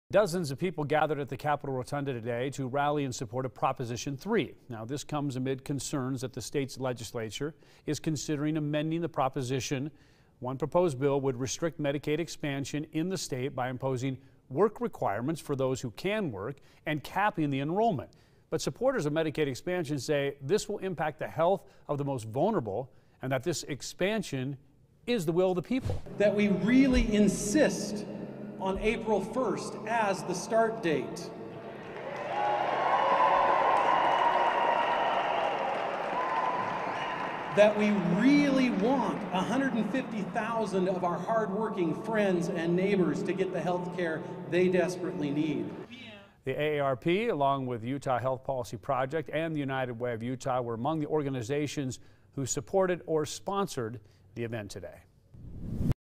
Utahns rally in support of Prop 3
‘No repeal, no replace': Utahns rally in support of Prop 3